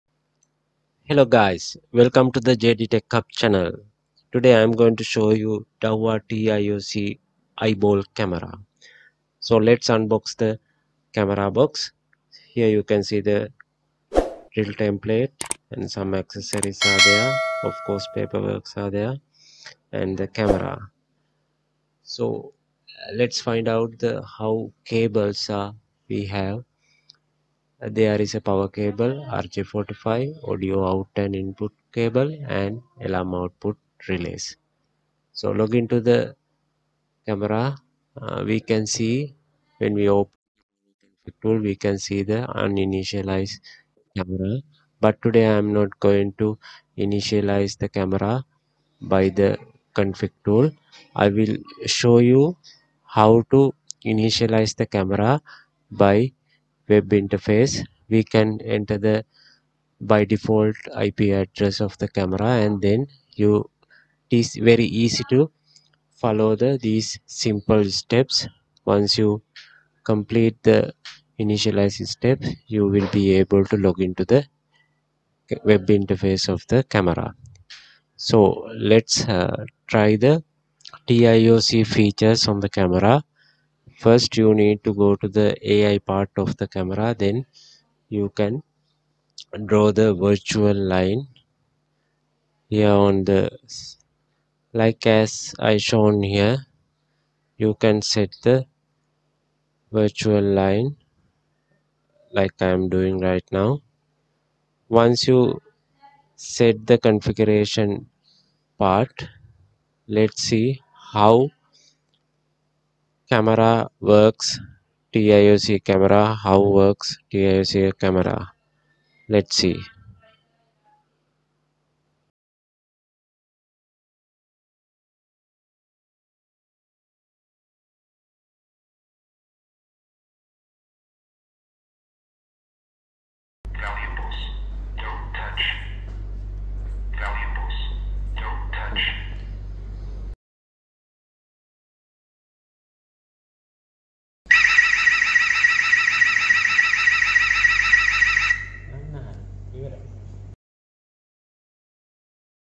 How Dahua TiOC camera initialized and work
In this video, I'm trying to show how Dahua TiOC camera initializes and how to use the active deterrence features of the camera.